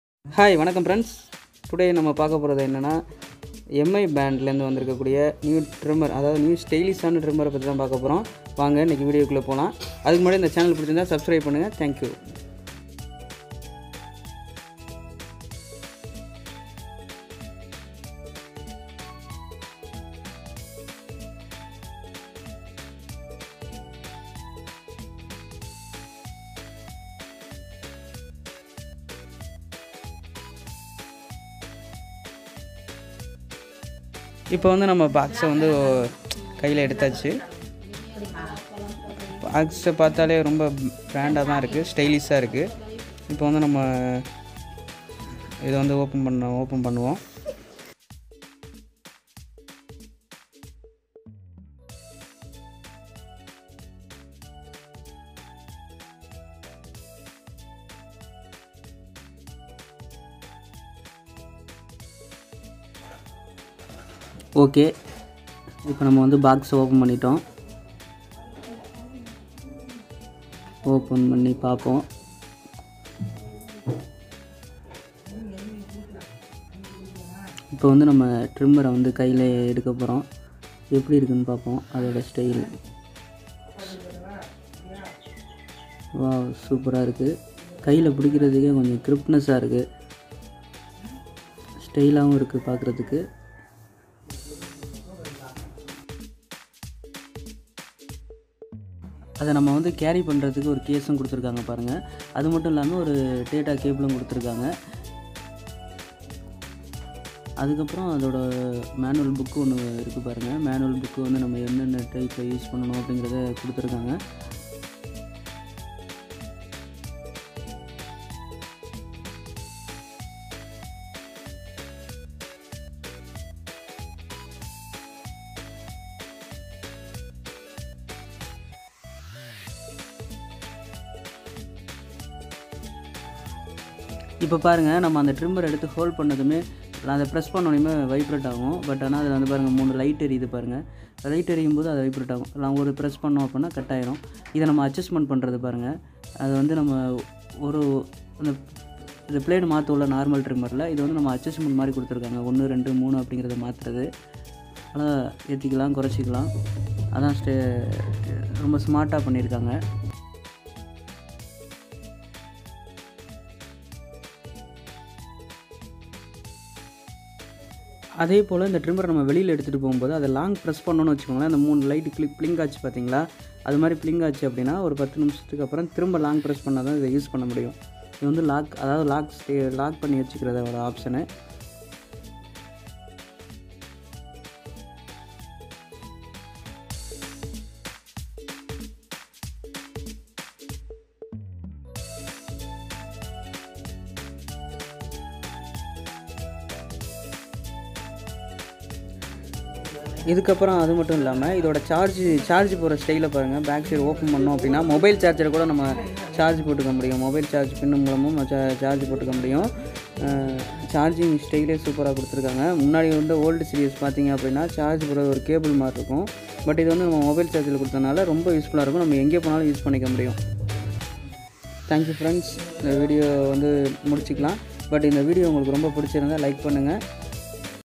MI BEARD TRIMMER 1C UNBOXING | MI TRIMMER LOW PRICE @999😍|TAMILMASTER
Blade Material: Self sharpening Stainless steel blades with skin friendly rounded tips

Length Settings: 40 length settings with 0.5mm precision with two combs

Run Time and Charging Time: Ultra powerful Battery with upto 90min of cordless usage with 2hour charge time

Easy to use: Versatile Corded and Cordless usage

IPX7 with a fully washable body for easy cleaning

Travel Safe with the Travel lock feature

Unique quad edge design to reach tough spots effortlessly

Warranty: One year pan-India warranty on all Manufacturing defects.

The trimmer blades come with prior lubricant coating. With usage, the lubricant might tend to dry up causing increased friction and resultant heating. It is recommended to use coconut oil to lubricate the underside of the blade if required

Box contents-Trimmer, Small comb, Long comb, travel pouch, cleaning brush and charging cable